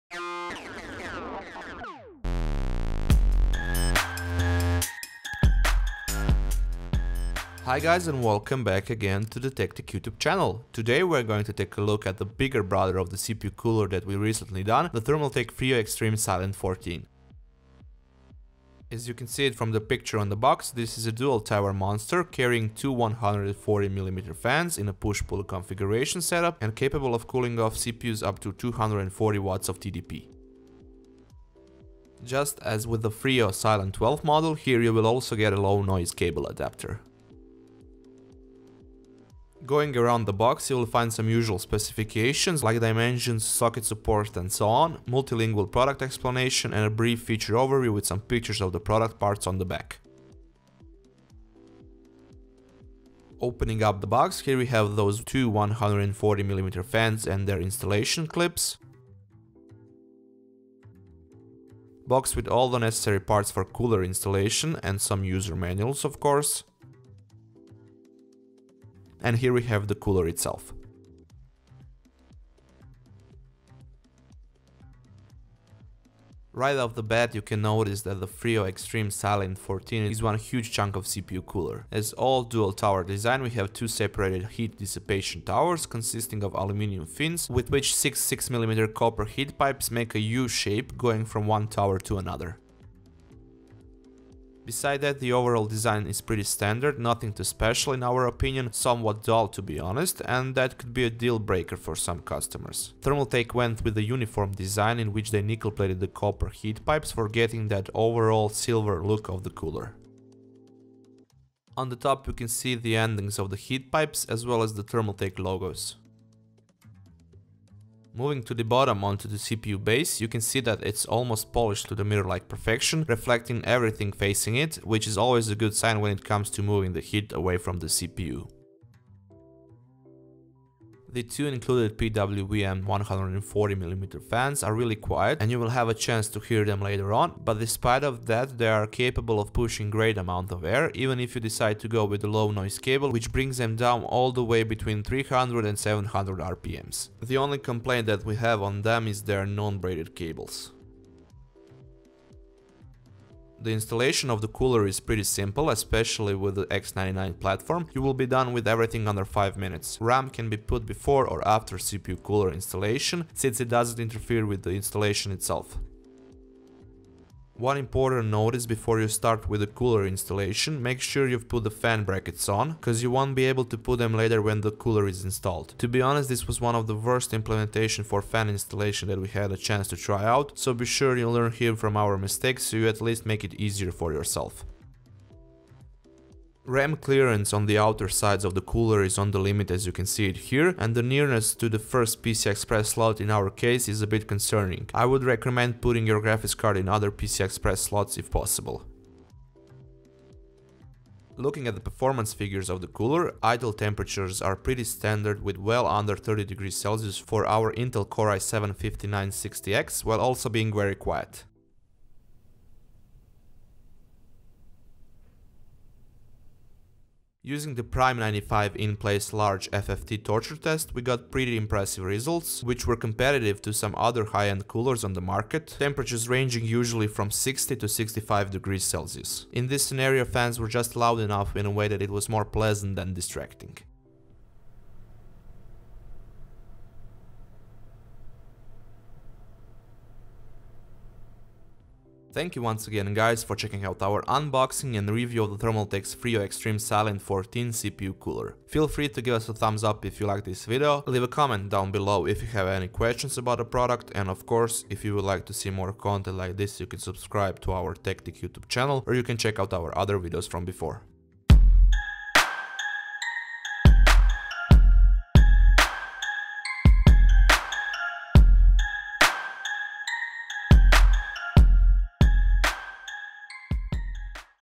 Thermaltake Frio Extreme Silent 14 Unboxing & Review
The Thermaltake Frio Extreme Silent 14 is one of the bigger models in the Thermaltake's Frio series of CPU coolers and today we're going to check out its performance and do an unboxing of it!